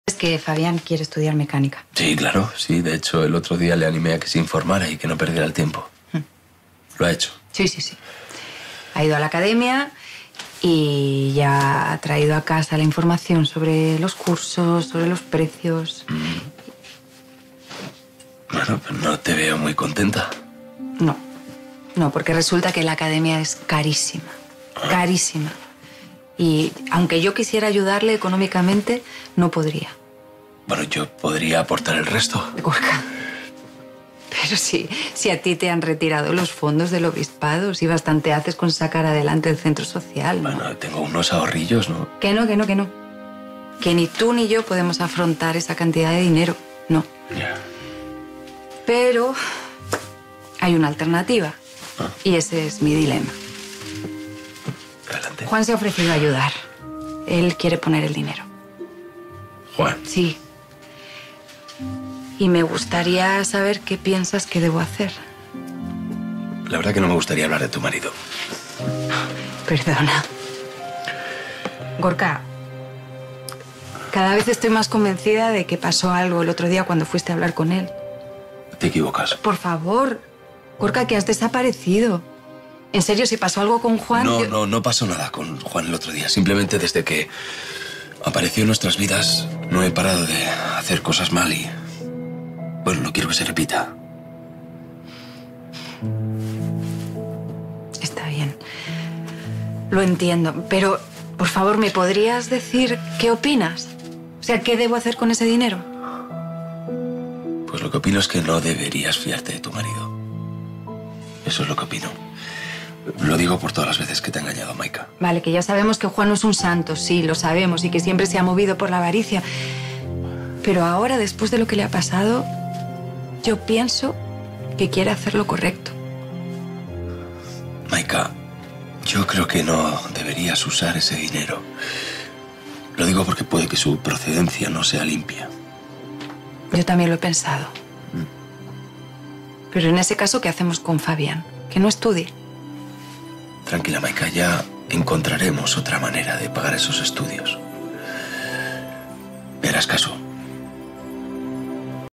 Maica sospecha que Gorka le oculta algo importante
La mujer busca al cura para pedirle consejo y se encuentra con una persona distante y misterioso.